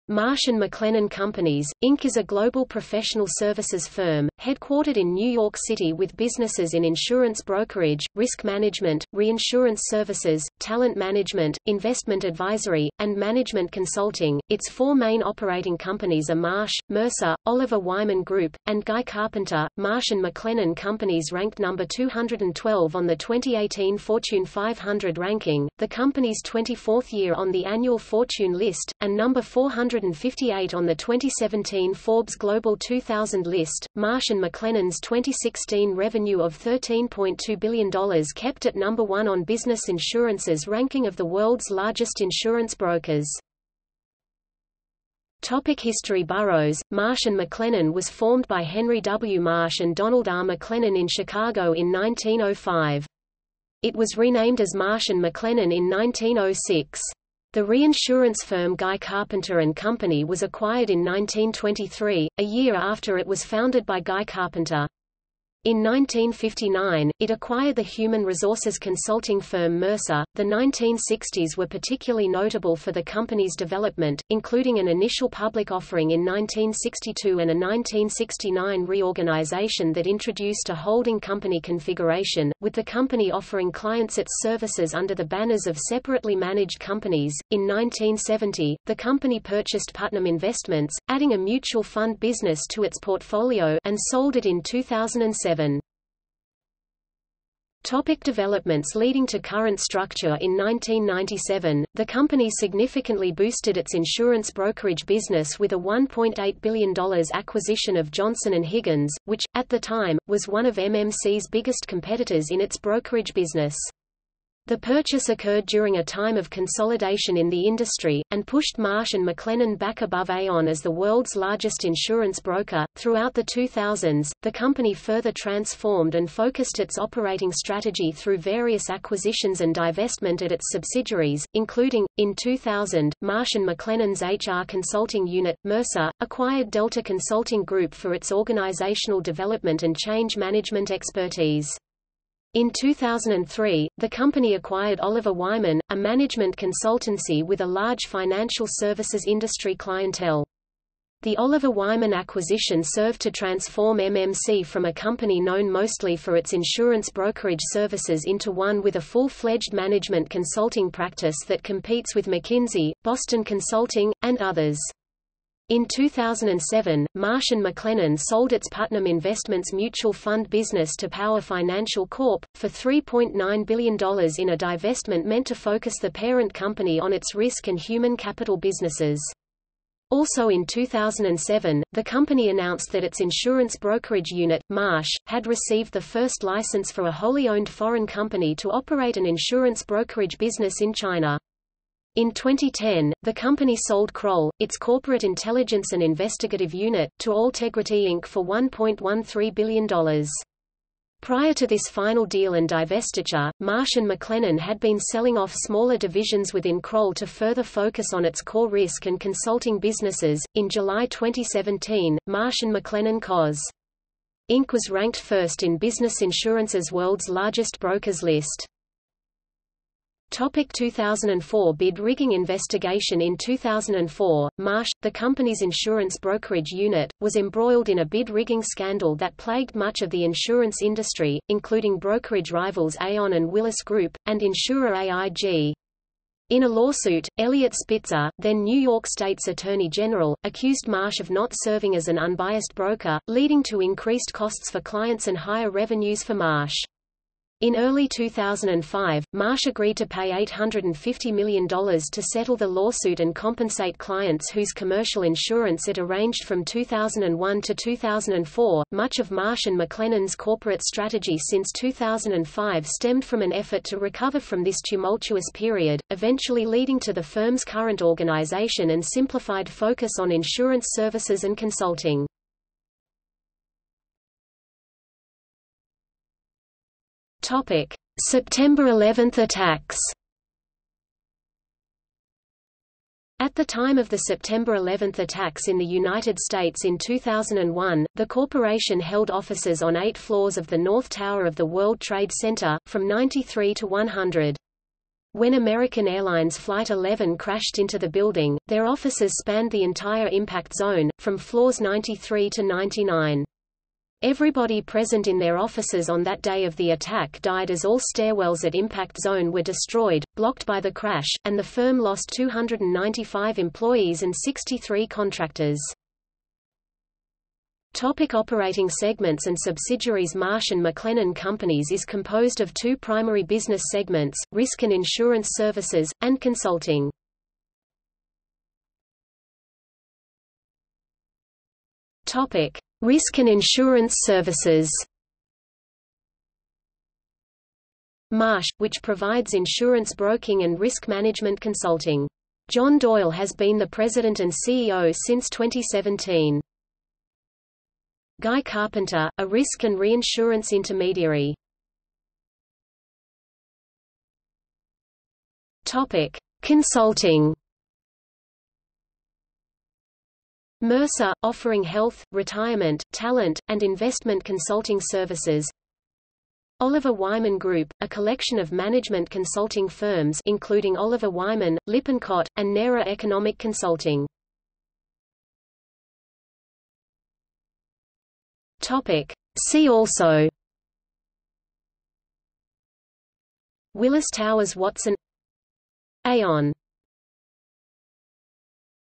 Marsh & McLennan Companies | Wikipedia audio article
00:00:45 1 History
00:01:37 1.1 Developments leading to current structure
00:03:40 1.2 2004 bid-rigging investigation
00:04:34 1.3 September 11 attacks
00:05:17 2 Operating segments and subsidiaries
00:05:29 2.1 Risk and insurance services
00:05:52 2.2 Consulting
00:06:15 3 See also

- Socrates

SUMMARY
Marsh & McLennan Companies, Inc. is a global professional services firm, headquartered in New York City with businesses in insurance brokerage, risk management, reinsurance services, talent management, investment advisory, and management consulting.Its four main operating companies are Marsh, Mercer, Oliver Wyman Group, and Guy Carpenter.Marsh & McLennan Companies ranked #212 on the 2018 Fortune 500 ranking, the company's 24th year on the annual Fortune list, and #458 on the 2017 Forbes Global 2000 List.Marsh & McLennan's 2016 revenue of $13.2 billion kept it #1 on Business Insurance's ranking of the world's largest insurance brokers.